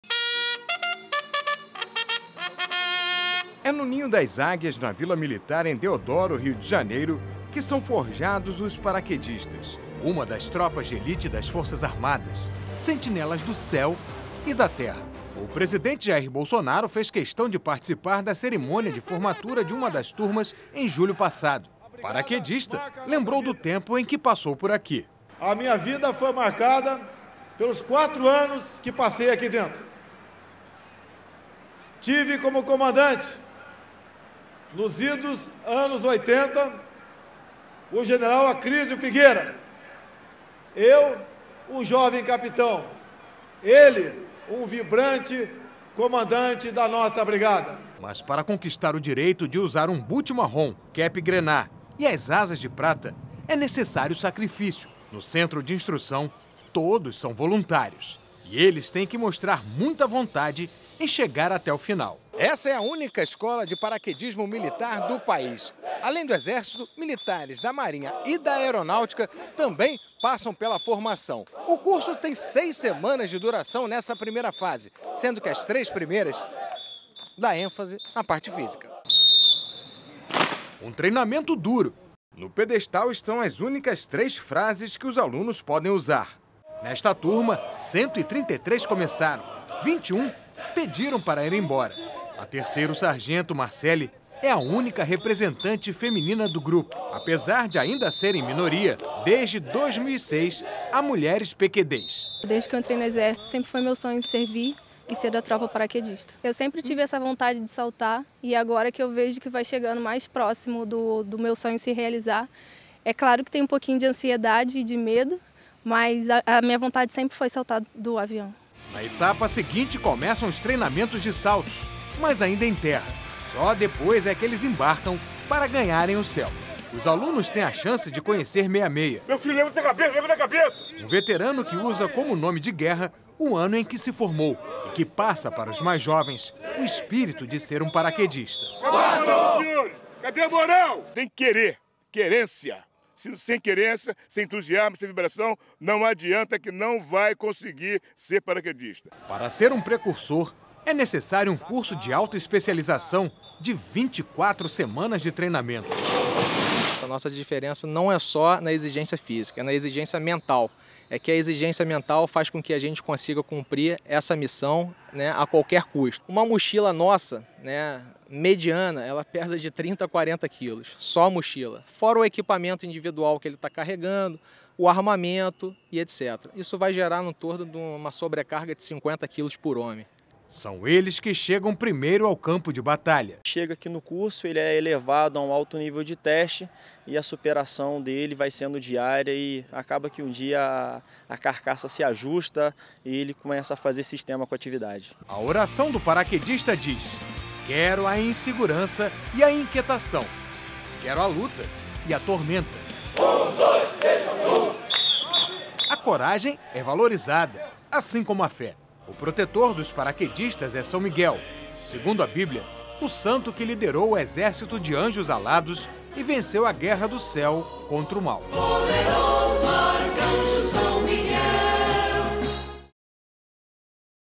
Como é o treinamento para se tornar paraquedista do Exército
Na segunda reportagem especial sobre a Brigada Paraquedista do Exército Brasileiro, o repórter Marcelo Castilho mostra como é o curso de formação para se tornar integrante do chamado "Ninho das Águias" e o diferencial dos militares que são os primeiros a chegar em locais de combate.